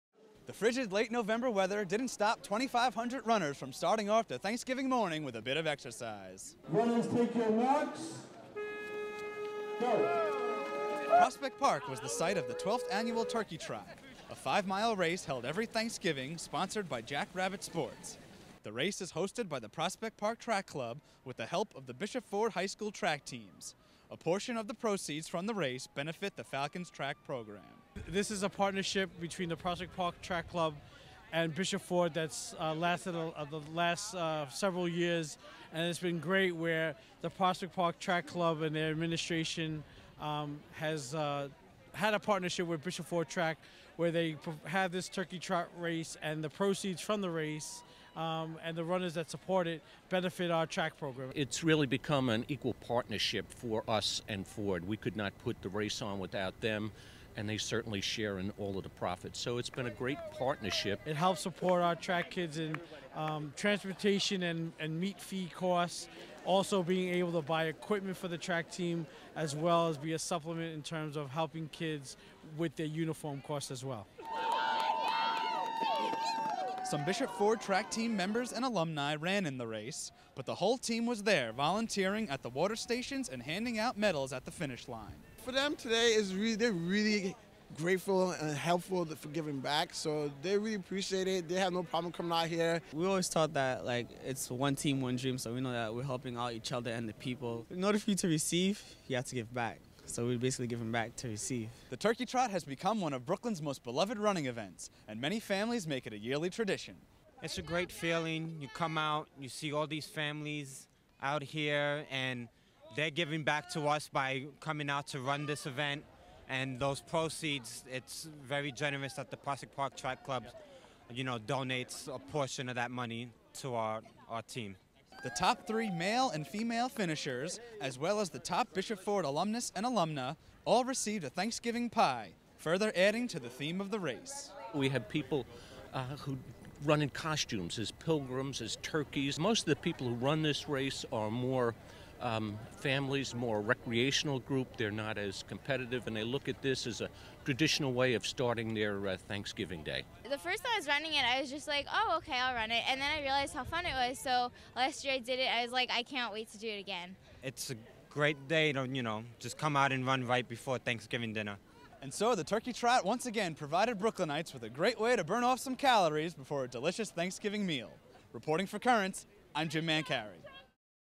Annual Turkey Trot Supports Bishop Ford Track Teams
The Prospect Park Track Club hosted the 12th annual Turkey Trot five-mile race on Thanksgiving morning. A portion of the proceeds benefit the Bishop Ford H.S. track teams.